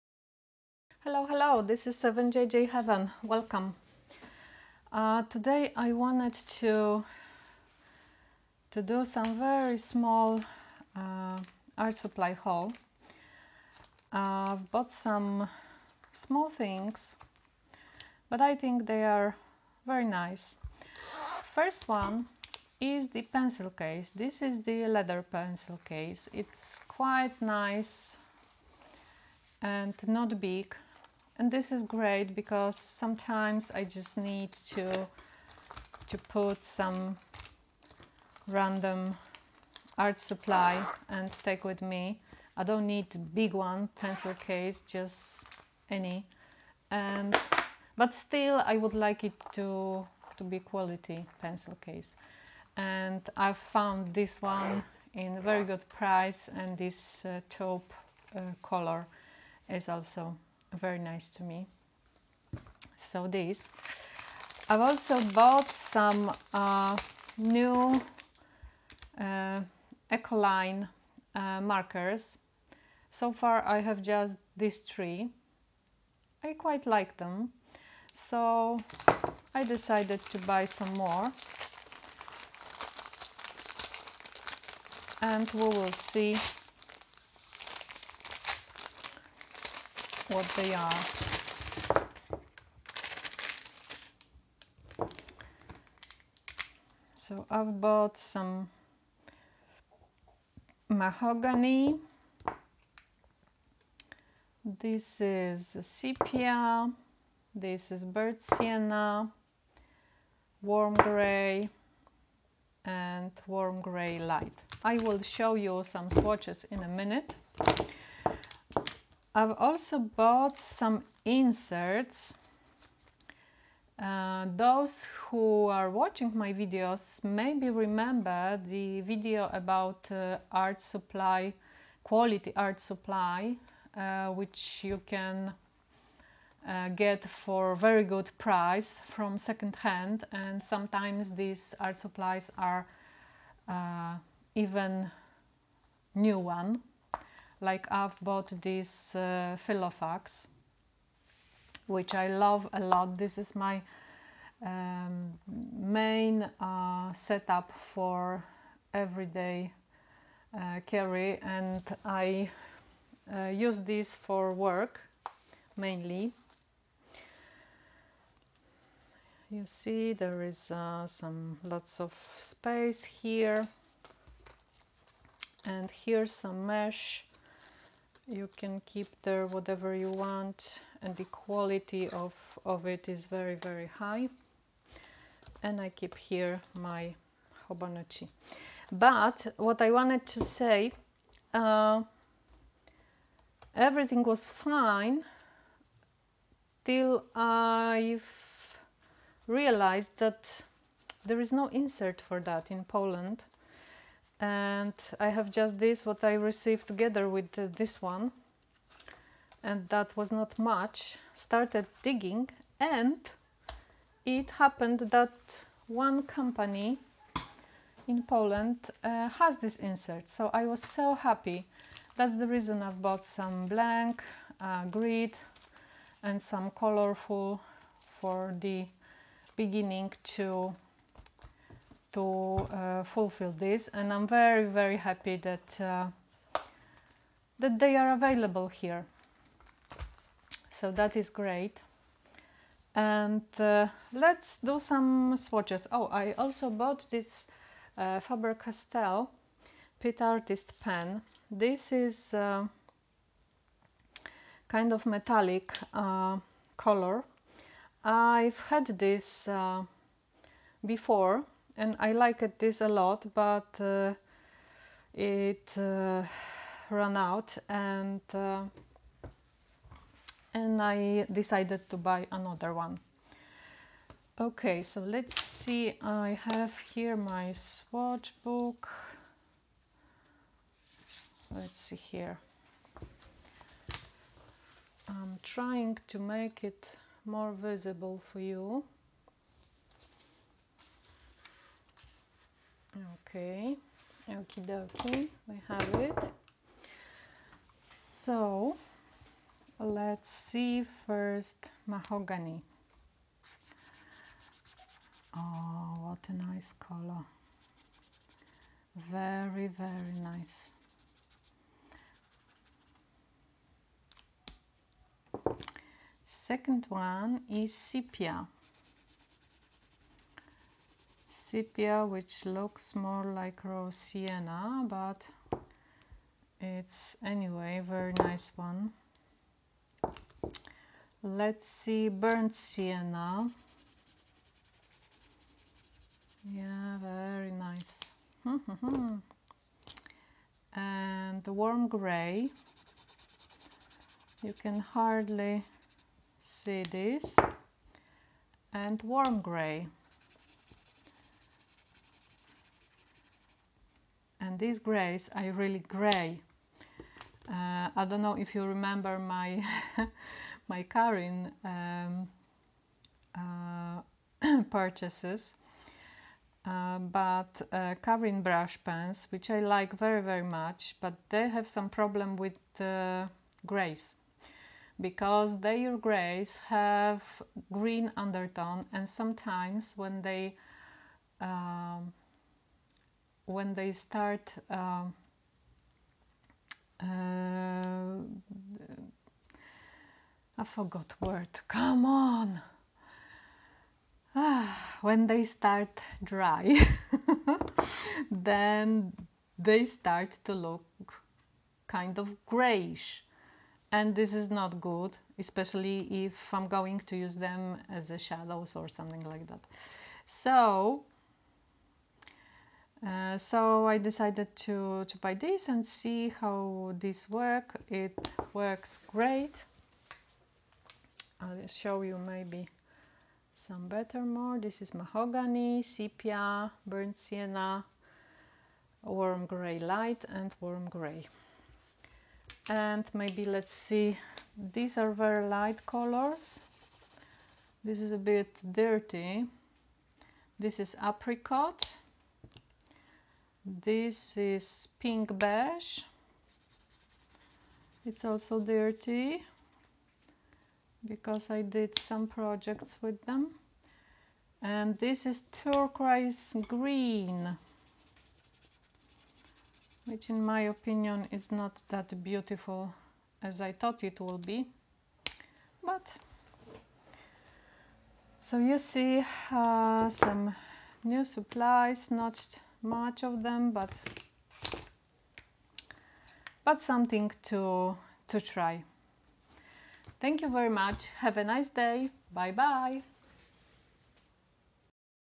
Stationary Small Haul – Inserts for Filofax Personal Size
Some small goodies: Ecoline brush pens, pencil case, inserts for Filofax personal size - currently my favorite planner.

It is always very encouraging to do more for you. There is lots of art stuff waiting for sharing with you.